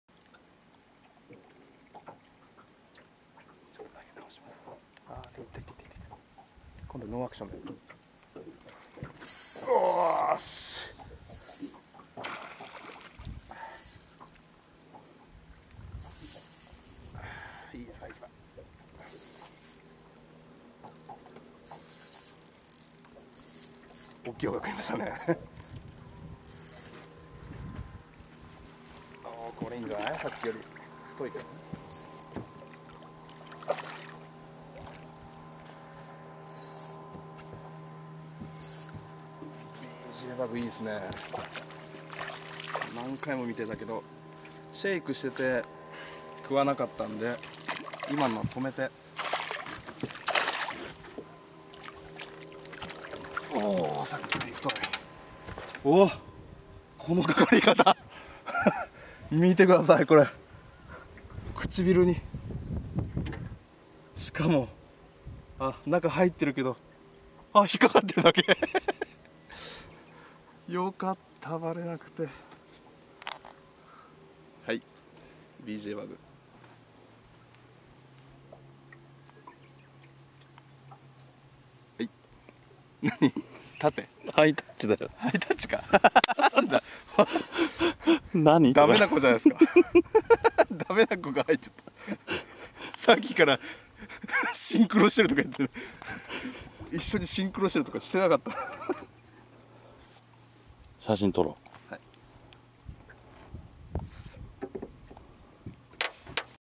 （５）野尻湖 虫 ２００９
エキサイティングな朝でした。
この日の状況を見ているとこれからの夏本番が楽しみです。
朝からピーカン無風、そんな時こそＢＪ　ＢＵＧです。